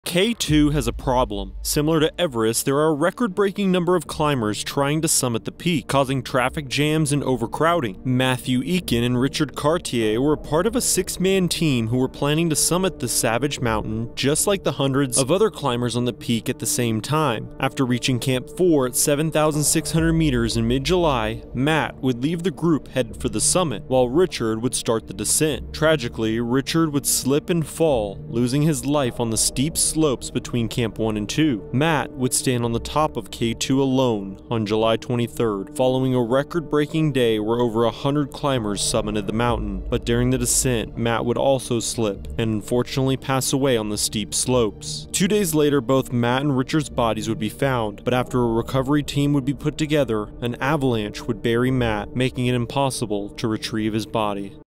Why K2 is becoming the next Everest
The tragic circumstances taking place on K2 this very moment...